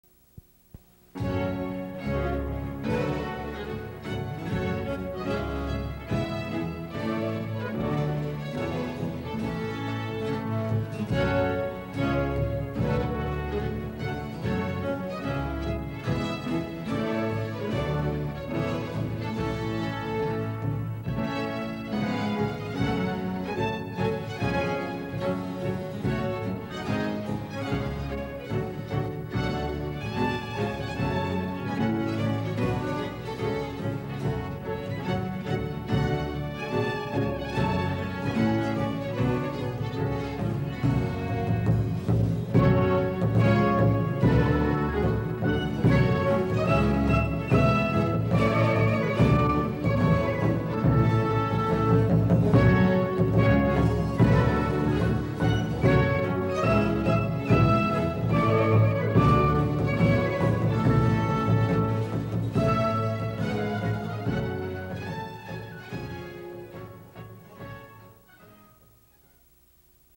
Man Bites Dog - Trailer
The assignment was to make a trailer, less than one minute and thirty seconds, using any music we chose and only that music, no audio from the film could be included.

Edited on a VHS deck to deck system. Jordi Savall is the composer of the musical track, somehow I thought a great french composer for a french film worked well together.